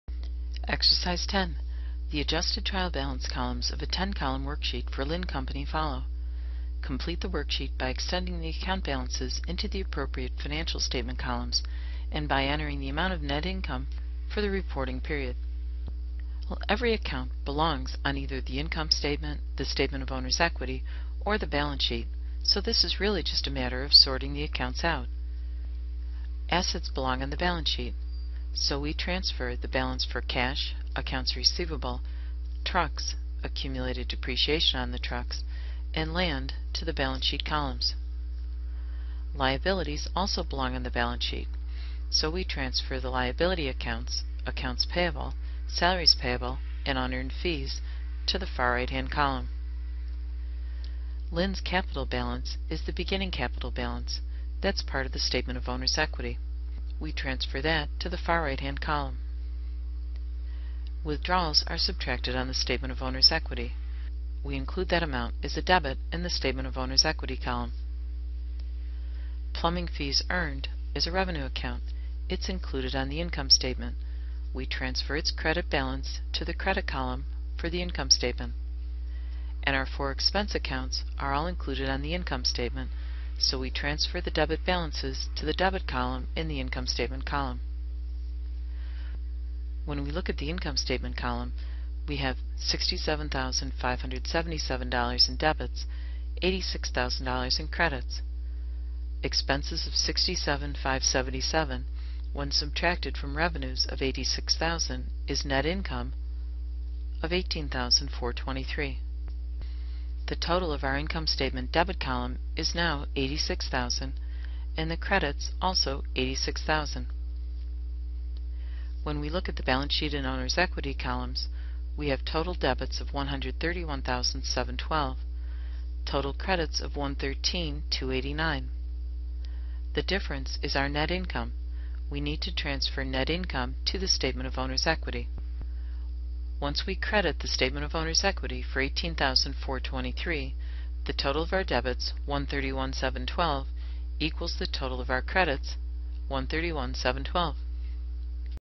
Extend Account Balances to Appropriate Column Exercise 4-10
Adjusment Trial Balance columns 10-column worksheet for Linn Company follow. Complete the work sheet by extending the account balance into the appropriate financial statement columns and by entering the amount of the net income for the reporting period.